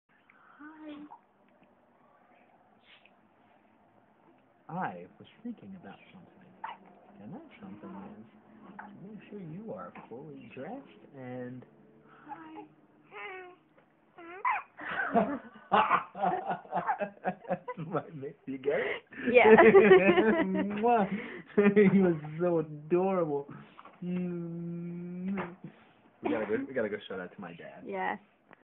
8 week old says "Hi"
My niece is only 8 weeks old and she has already said her first word!